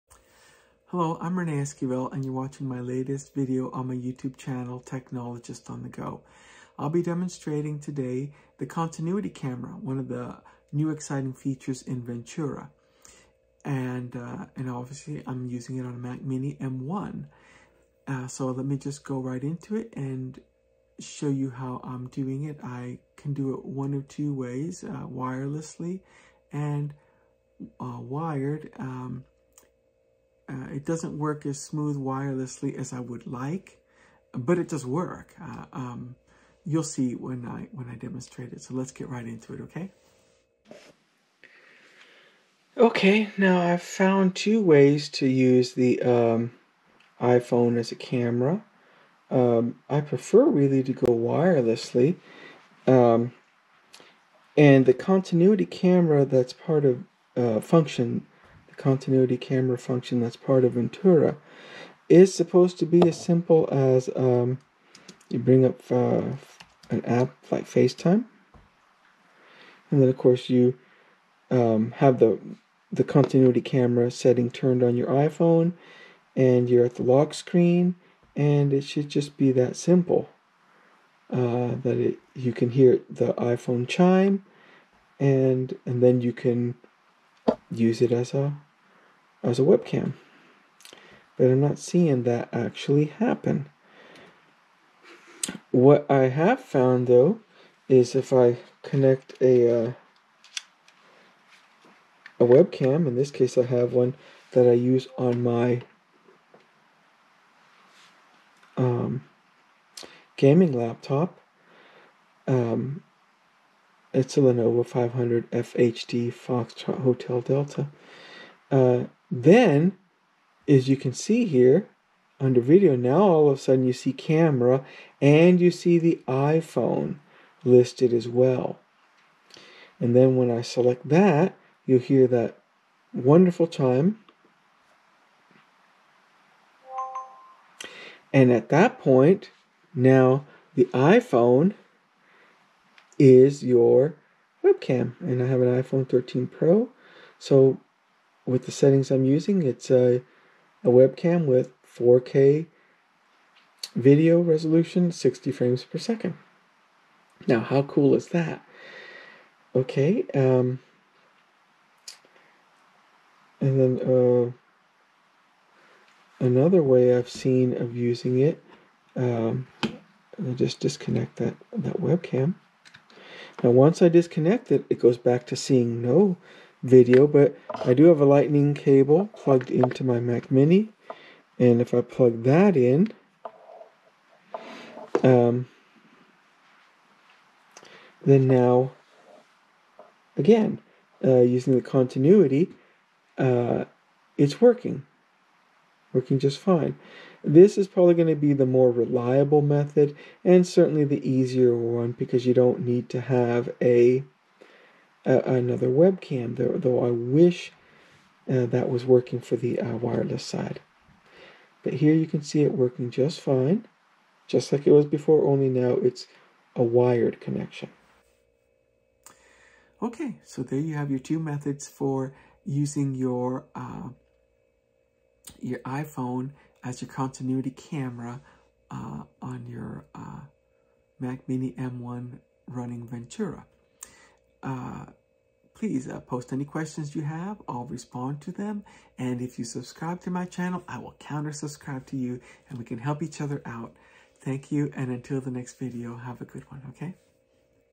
Ventura Continuity Camera on Mac Mini M1 to Use iPhone as a Wired or Wireless Webcam
As part of the ongoing videos on "Getting the Most Out of Your Mac Mini M1", this video shows how to use the new Ventura feature called "Continuity Camera" to use your iPhone as a wired or wireless webcam. Unlike the Apple laptops and iMacs, the Mac Mini M1s do not have a built-in webcam. For those remote workers or end-users wanting to video chat, continuity camera allows you to not only get a wired or wireless webcam, but to do so at the max resolution your iPhone supports. In this video, the iPhone 13 Pro provides 4k video at 60 fps.